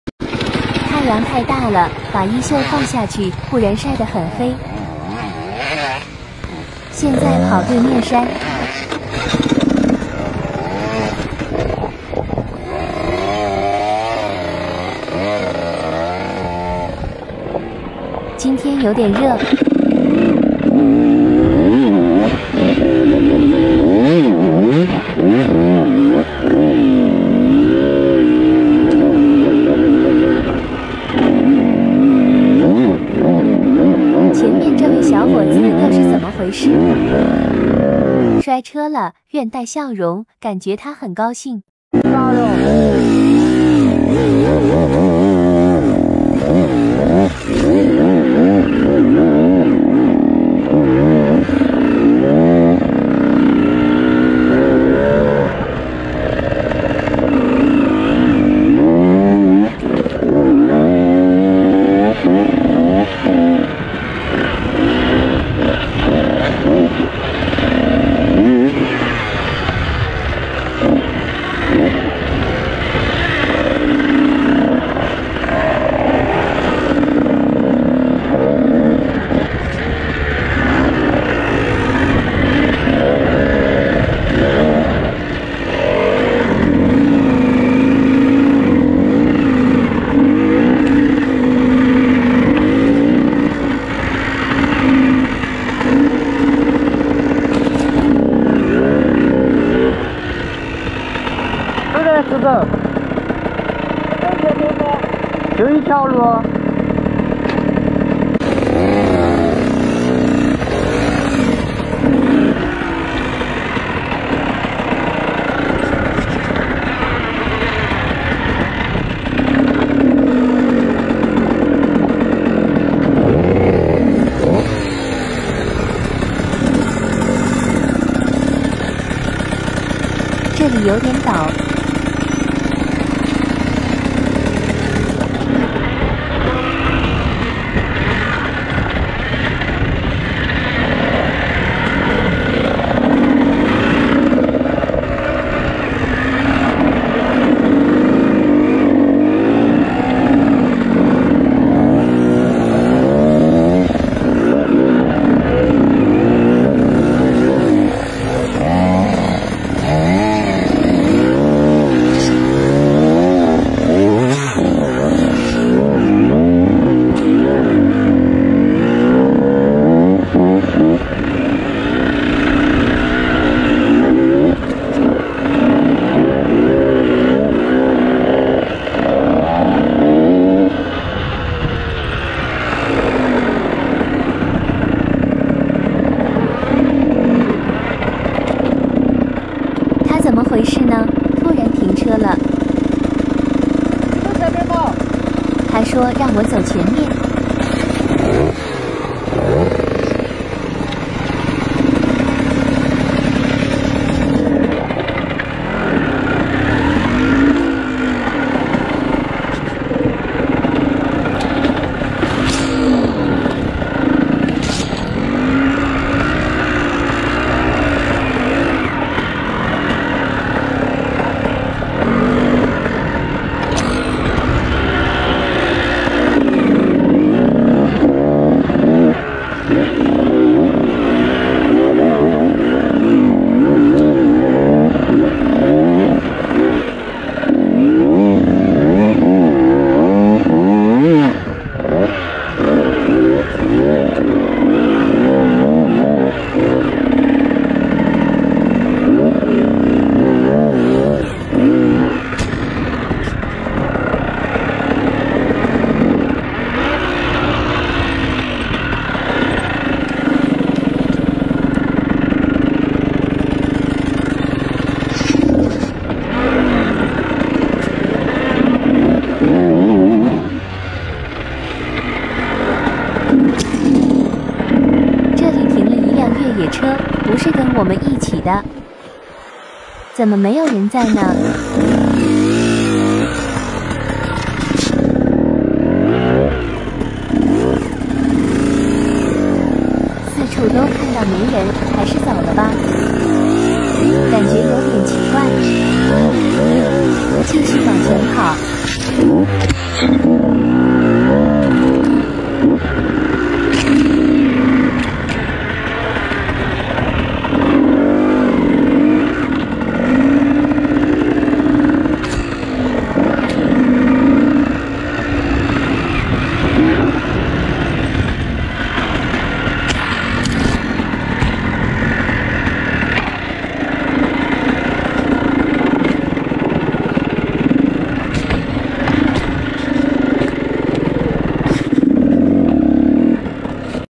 My Video80Jarvis | Abestone Hard Enduro 2021 | Super Final Onboard | WHEC
Sports team travel on the most difficult and danderous road through the rocky valley . Road to pass throuhg dense forest/ canyon /  Please click and ''SUBSCRIBW '' scroll have new videos